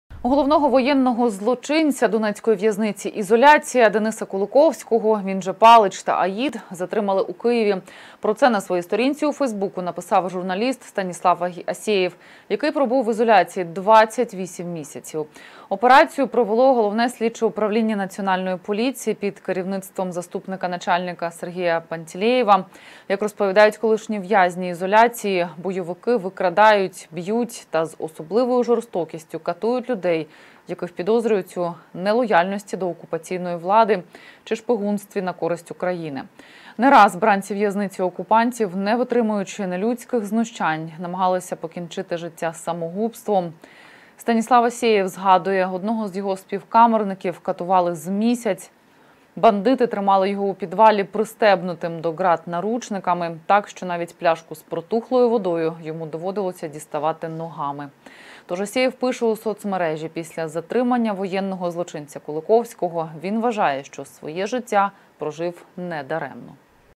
Затримали керівника донецької в'язниці, де катували українських патріотів
Головного воєнного злочинця донецької в’язниці «Ізоляція» Дениса Куликовського, він же Палич і Аїд, затримали у Києві. Про це на своїй сторінці у фейсбуку написав журналіст Станіслав Асєєв, який пробув в «Ізоляції» 28 місяців. Операцію провело головне слідче управління Національної поліції під керівництвом заступника начальника Сергія Пантелєєва. Як розповідають колишні в'язні «Ізоляції», бойовики викрадають, б'ють і з особливою жорстокістю катують людей, яких підозрюють у нелояльності до окупаційної влади чи шпигунстві на користь України. Не раз бранці в'язниці, не витримуючи нелюдських знущань, намагалися покінчити життя самогубством.